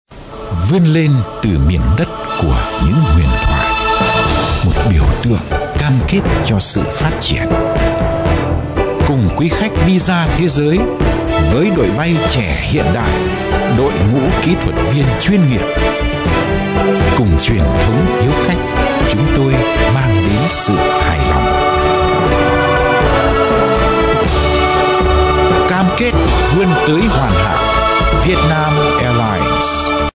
Vietnam Airlines Commercial Ads on TV [Vietnam version]
Chúng tôi cam kết đem đến cho khách hàng những dịch vụ tốt nhất :) Đến với Vietnam Airline để trải nghiệm những điều thú vị

We offer all service that you demand for a luxury flight :) Come with us and enjoy a fulfill experience at Vietnam